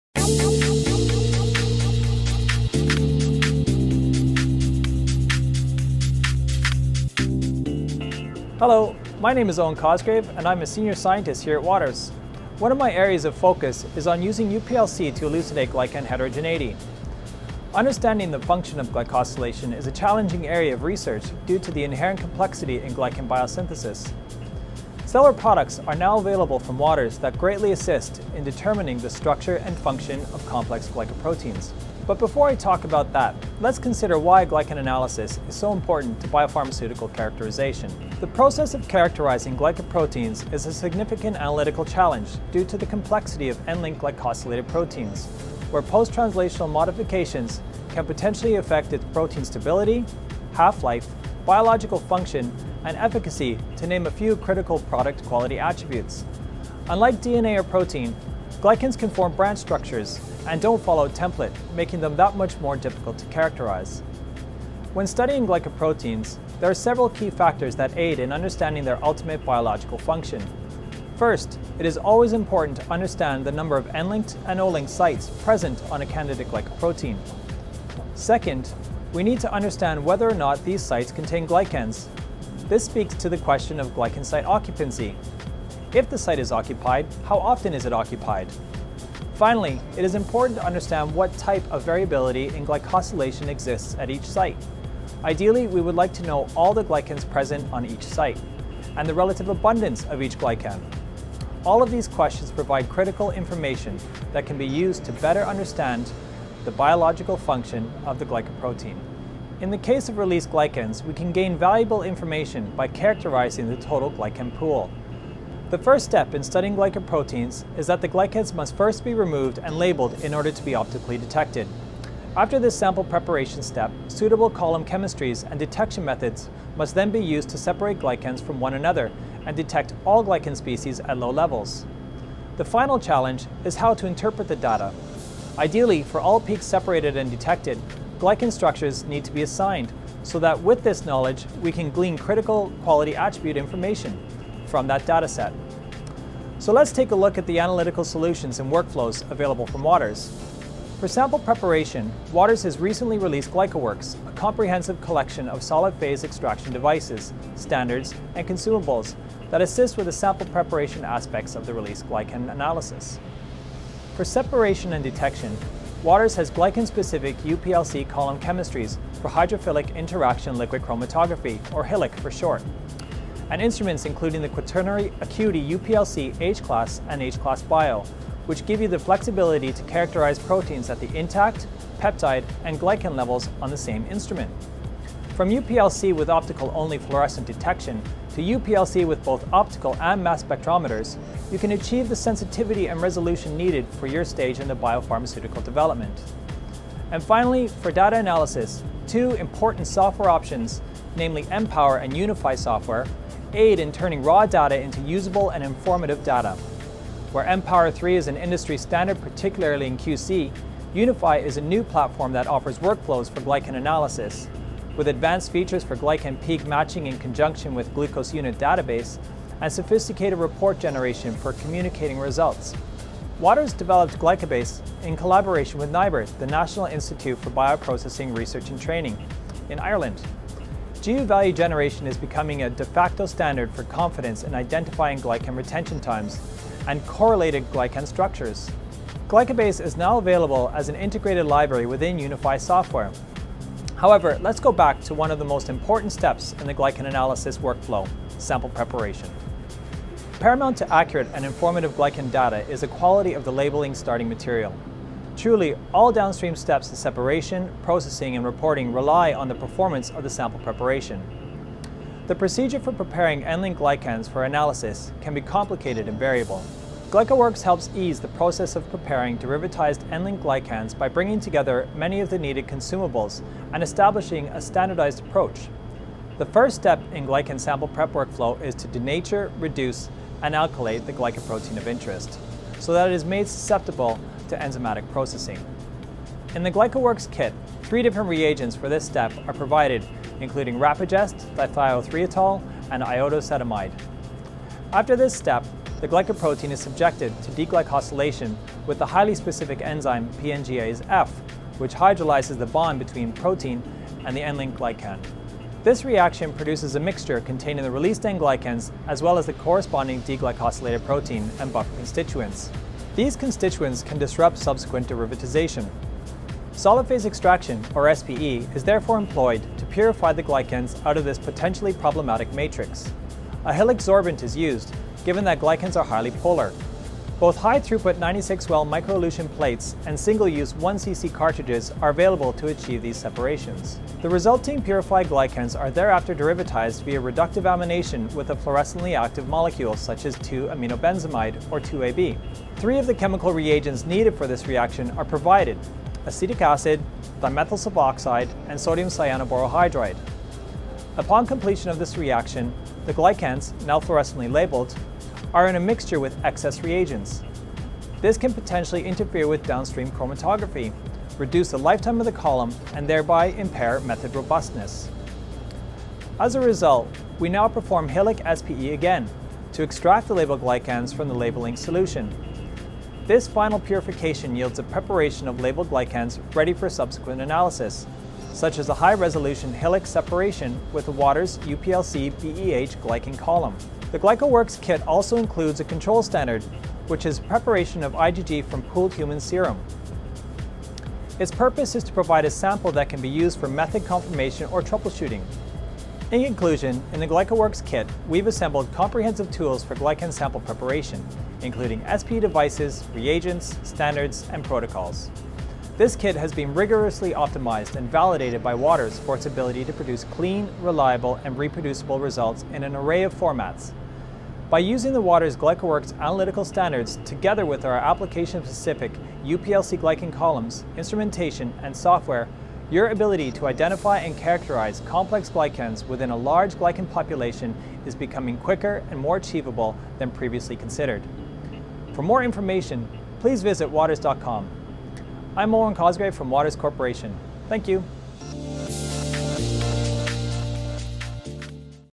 A Systematic Approach to Glycan Sample Prep and UPLC Analysis
In this video, Eoin Cosgrave, Waters Corporation, discusses the use of UPLC to elucidate glycan heterogeneity. The process of characterizing glycoproteins is a significant analytical challenge due to the inherit complexity of N-linked glycosylated proteins, where post-translational modifications can potentially affect protein stability, half-life, biological function, and efficacy. Unlike DNA or protein, glycans can form branch structures, and don't follow a template, making them that much more difficult to characterize. Waters Corporation has developed, the GlycoWorks Kit, to aid in glycan sample preparation, including SPE devices, reagents, standards, and protocols. By using the Waters GlycoWorks analytical standards together with Waters' application-specific UPLC glycan columns, instrumentation, and software, your ability to identify and characterize complex glycans within a large glycan population is becoming quicker and more achievable than previously considered.